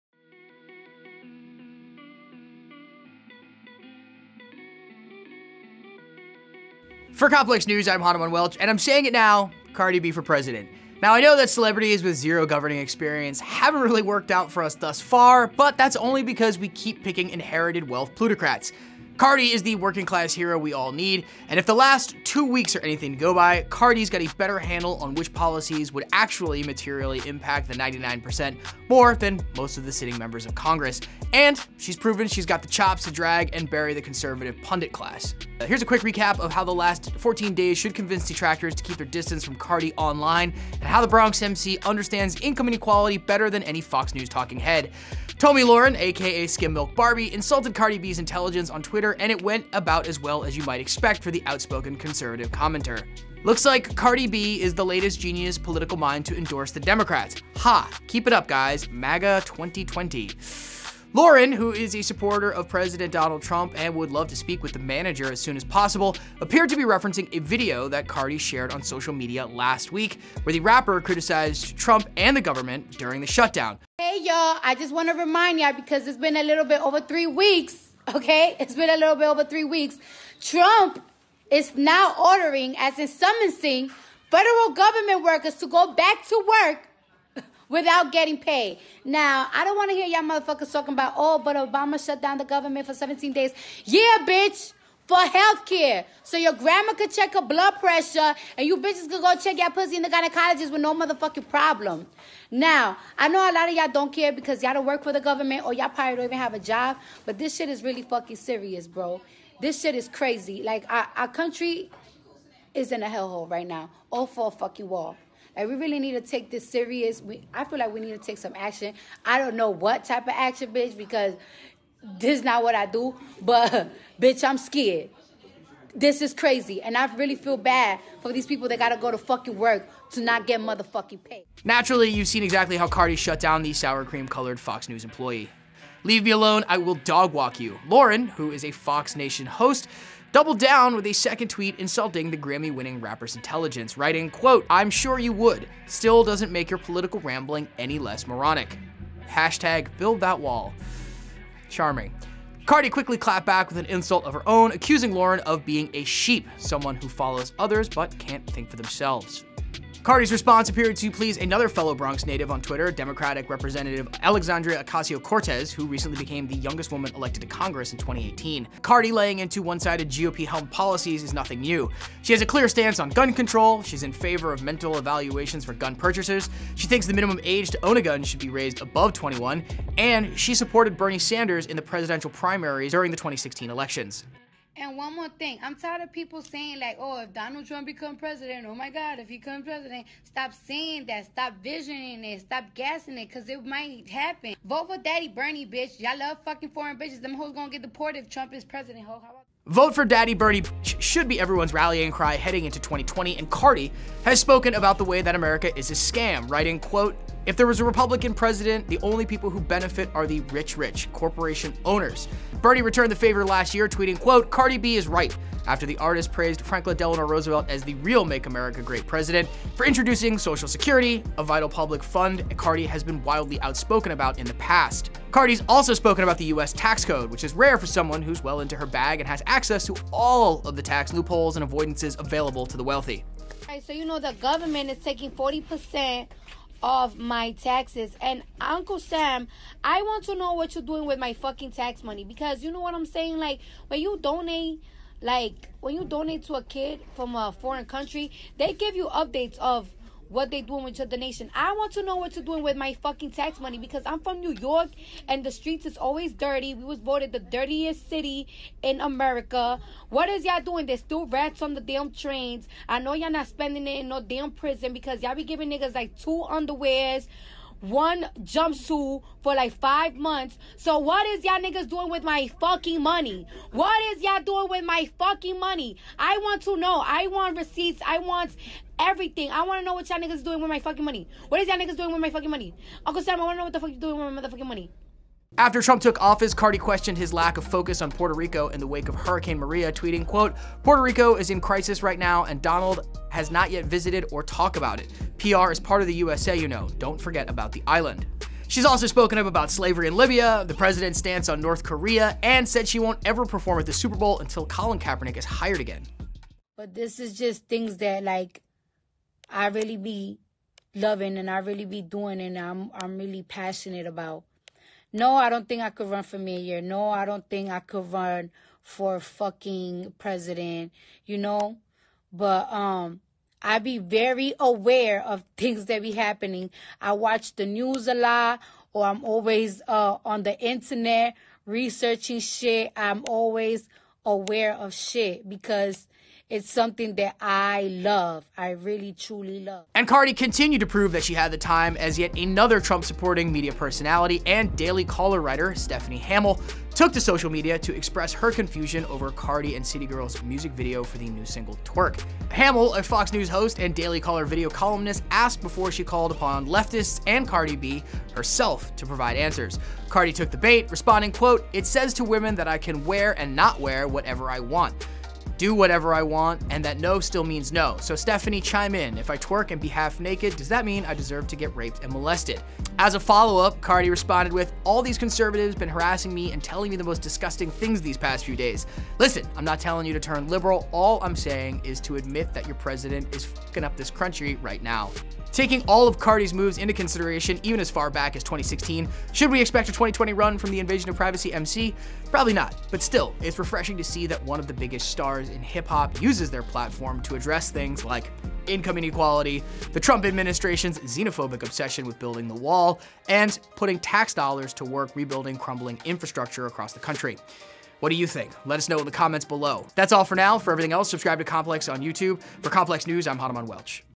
Cardi B Has Some Interesting Opinions When It Comes to U.S. Politics
Cardi B has spent the last two years proving she’s knows her shit when it comes to politics